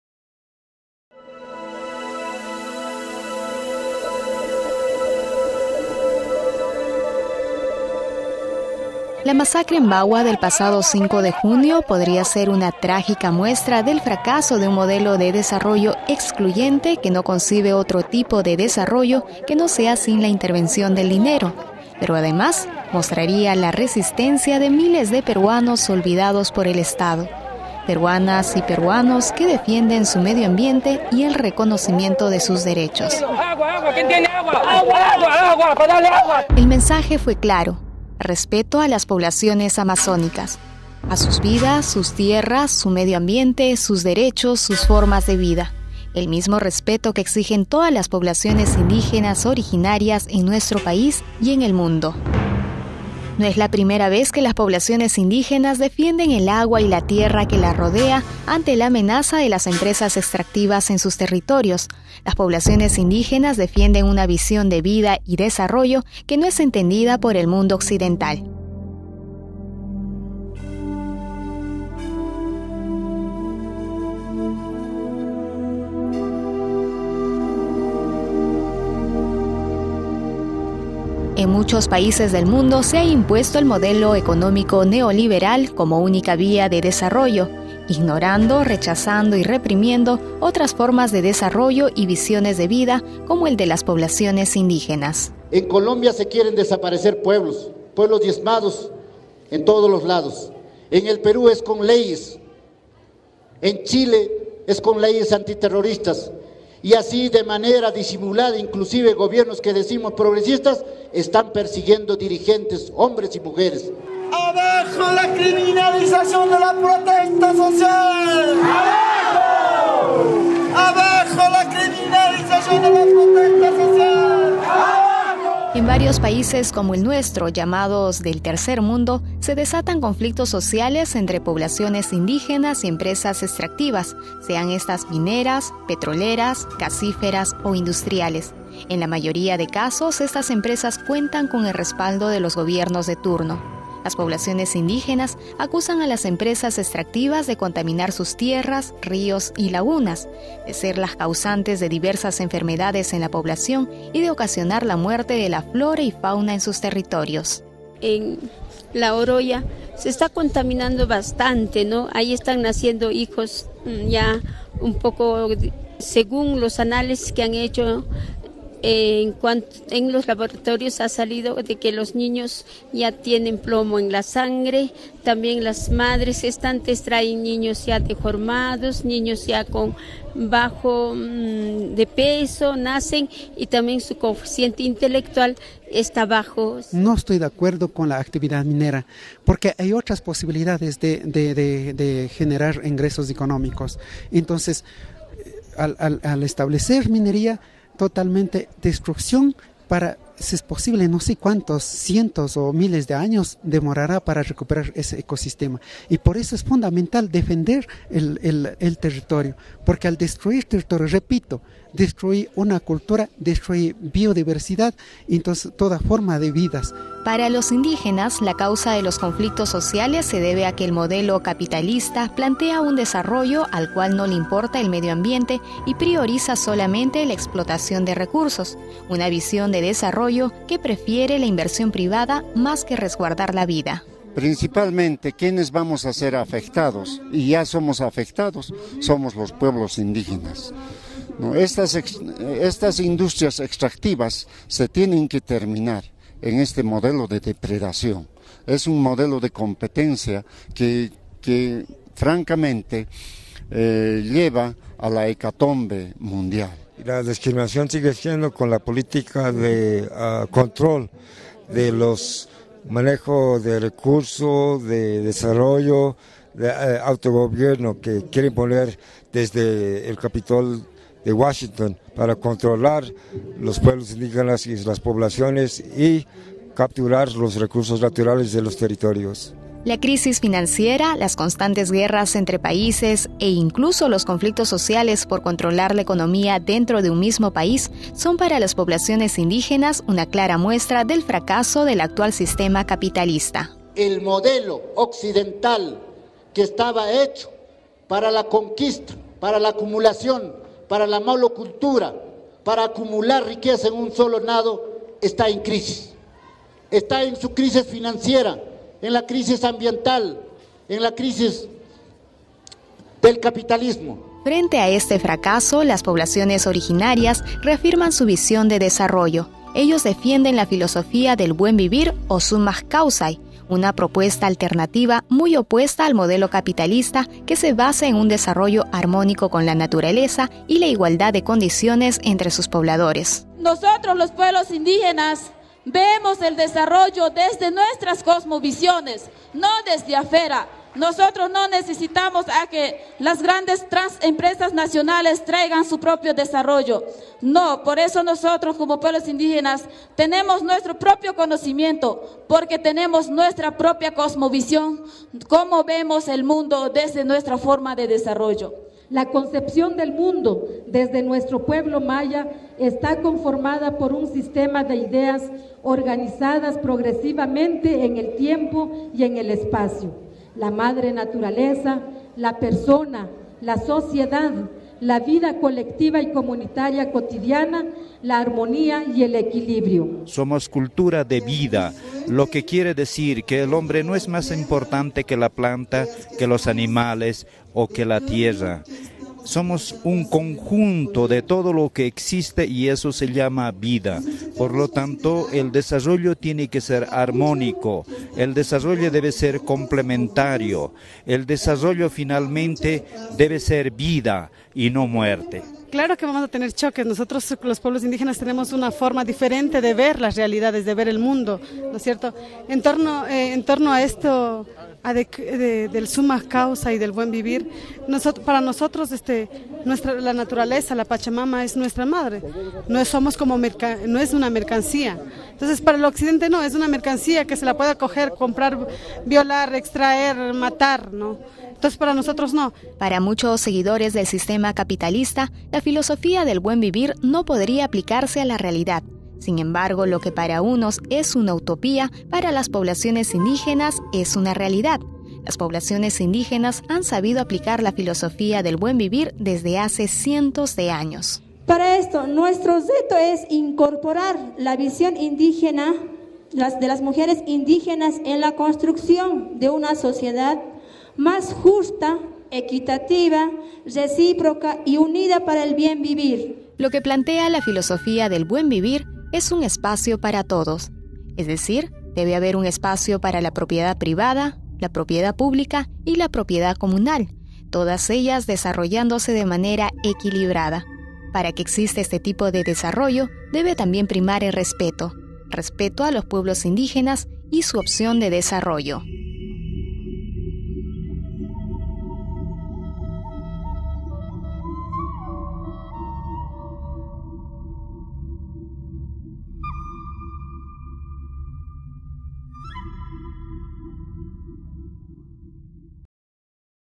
Súmac Káusai (Buen Vivir): propuesta alternativa de desarrollo (Reportaje)
La masacre en Bagua del pasado 5 de Junio, mostraría el fracaso del sistema capitalista, un modelo de desarrollo excluyente, que no concibe otro tipo de desarrollo que no sea sin la intervención del dinero. Pero además, mostraría la resistencia de miles de peruanos olvidados por el Estado, que defienden su medio ambiente y el reconocimiento de sus derechos.

Ellos piden Respeto a las poblaciones amazónicas. A sus vidas, sus tierras, su medioambiente, sus derechos, su forma de vida. El mismo respeto que exigen todas las poblaciones indígenas, originarias en nuestro país y en el mundo.

Las poblaciones originarias reafirman su visión de desarrollo. Defienden y plantean la filosofía del Buen vivir o Súmac káusai. Una propuesta alternativa muy opuesta al modelo capitalista, que se basa en un desarrollo armónico con la naturaleza y la igualdad de condiciones entre sus pobladores.

ENTREVISTADOS:
Miguel Palacín  CAOI  Perú
Feliciana Amado  CONACAMI - Perú
César Pilataxi  CONAIE - Ecuador
Tomas Huanaco  CONAMAQ - Bolivia
Túpac Acosta  Indígena - EE. UU.
Rosilda Nunta - AIDESEP - Perú
Juana Batzibal - CALDH - Guatemala
Isidro Mamani  Indígena - Bolivia
Patricia Yallico  ECUARUNARI - Ecuador
Nacy Chila  CONAMAQ  Bolivia